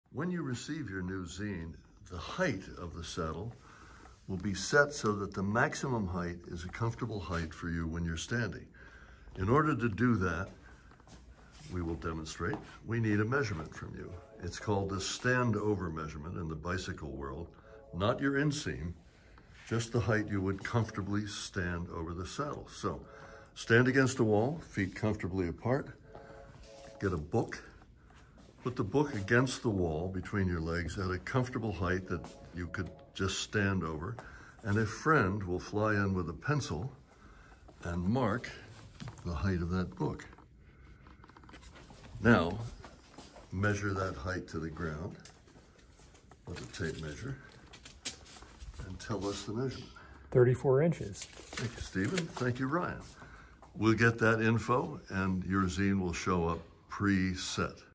14_b_Stand-Over Height Measurement
Measuring the Standing Height for the ZEEN adjustment can be accomplished using a tape measure, book and pencil along a wall location.  It is best to have a friend or assistant help with this process.  It is also important for the ZEEN user to wear shoes during this measurement process.

1. The ZEEN user should stand flat-footed with their back against the wall, and feet spread shoulder width apart
2. Place a book between the ZEEN user’s legs and raise it up as if they are sitting on the book binding.
3. While the book is position, ask an assistant to mark the wall at the top edge of the book binding.
4. Lastly, take a measurement from this mark on the wall to the floor.

This measurement will help our team adjust the ZEEN specifically for that user.  We will provide instructions for refining this maximum seat height adjustment with the ZEEN delivery.